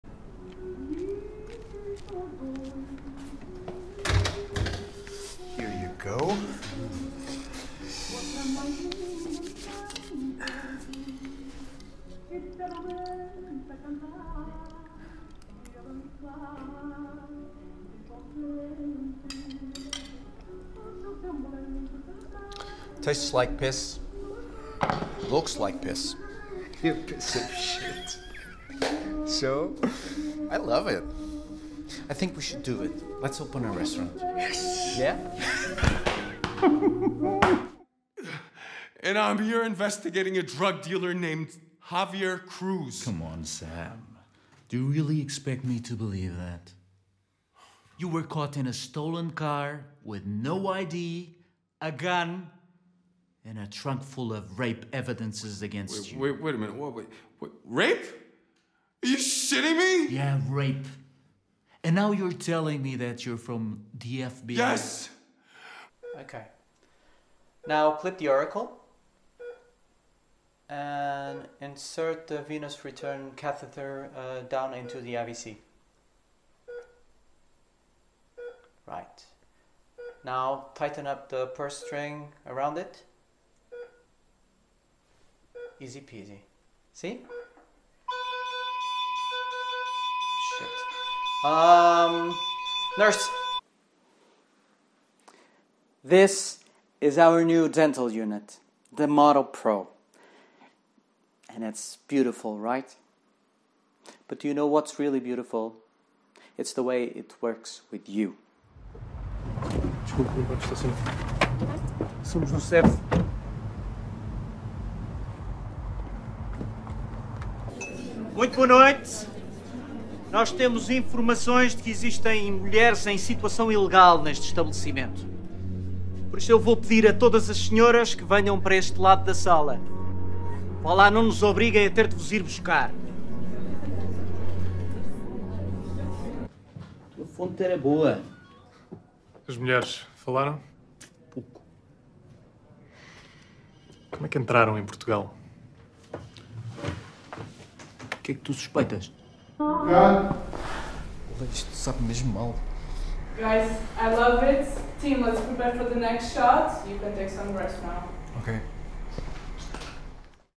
Henrique Gomes Showreel HD 1080p
Several scenes and takes from TV and cinema